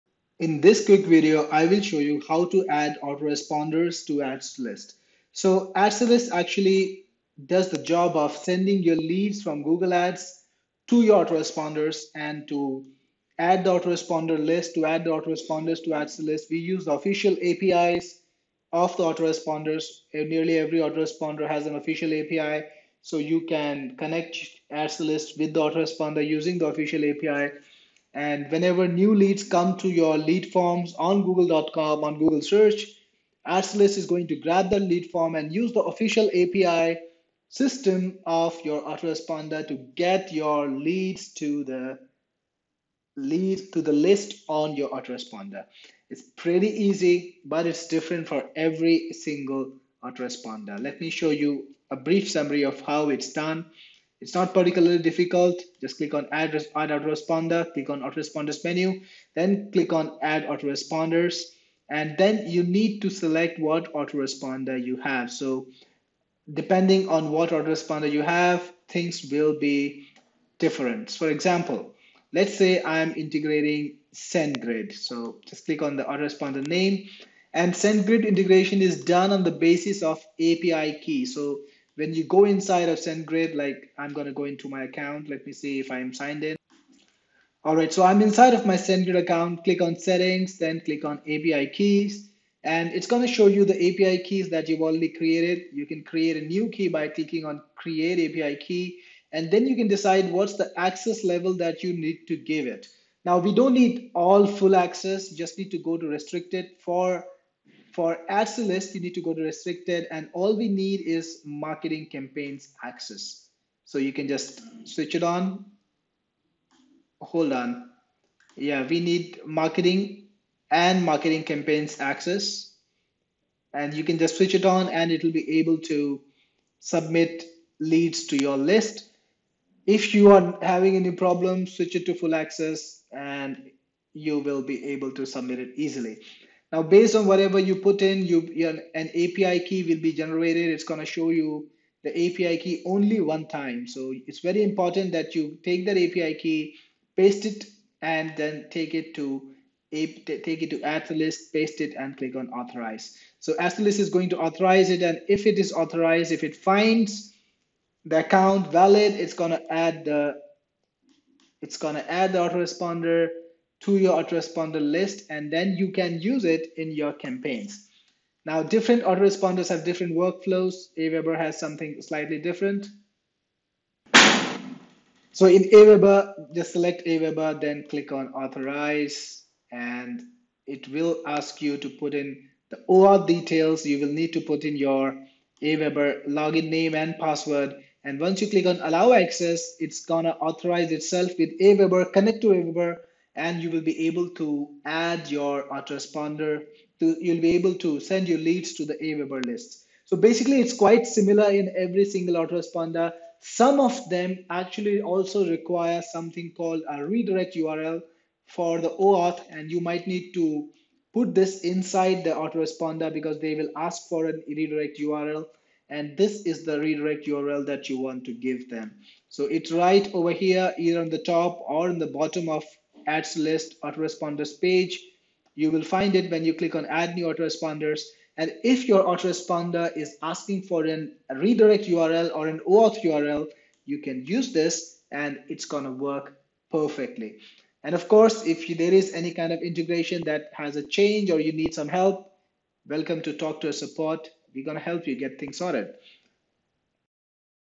Adding an autoresponder to Ads2List
Find out how to add autoresponders to Ads2list. Ads2list is a powerful tool that lets you send your leads from Google Ads straight into your autoresponder on automation.

You can connect autoresponders using their official APIs. Ads2list uses this API system to send leads to your lists in autoresponders.

The steps to add each autoresponder is different. Take a look at this video to find out more.